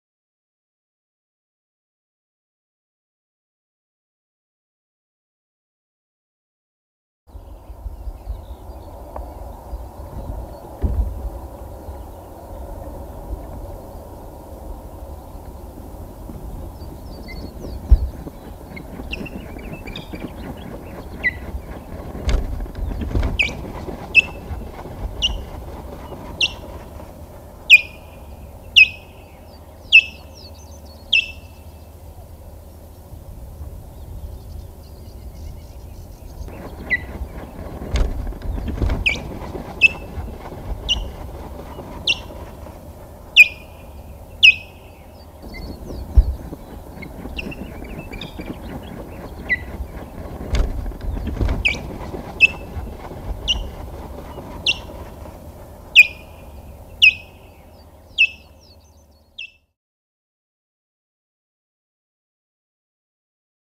Blue 24 - Touchdown
9th June 2015

 Love her or hate her, you've got to hand it to Blue 24.

Glesni's cousin got so close to the nest this morning, she actually touched down! The remarkable thing is, Monty and Glesni were relatively relaxed about the whole thing.